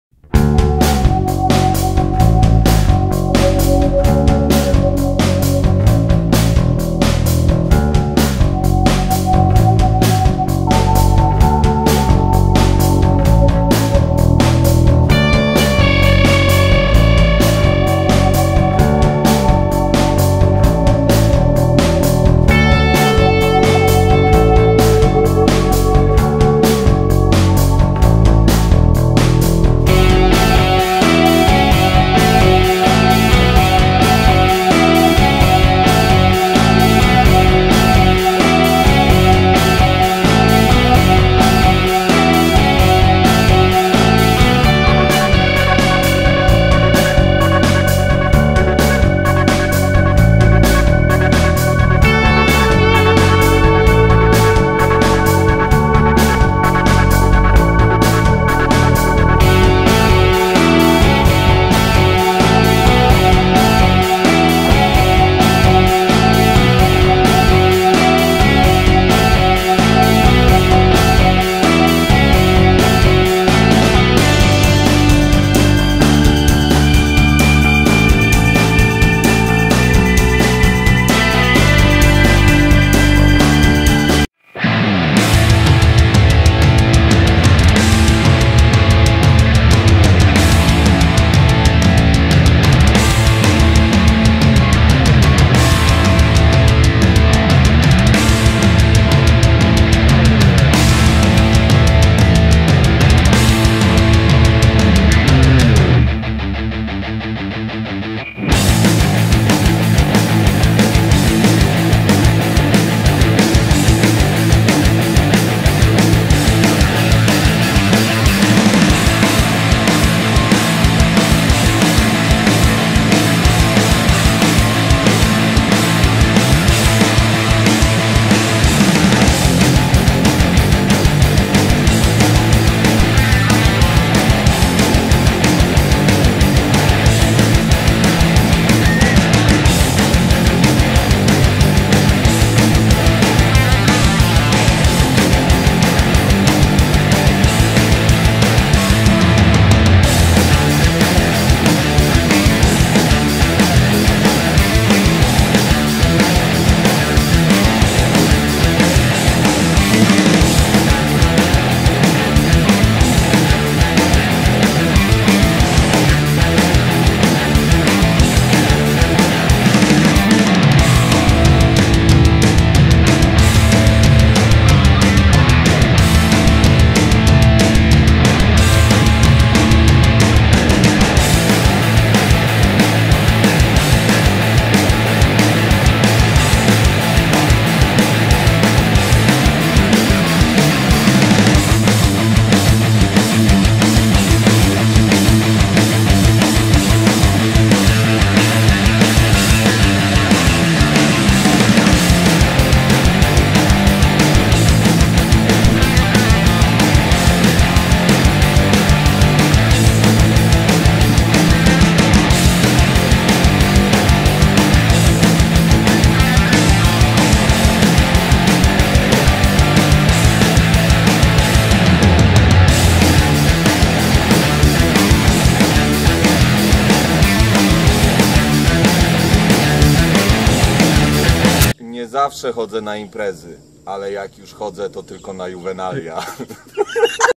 Juwenalia 2014 - Sunrise Farm - Hinckley, IL
Short video of last year's Juwenalia (2014) @ Sunrise Farm in Hinckley, IL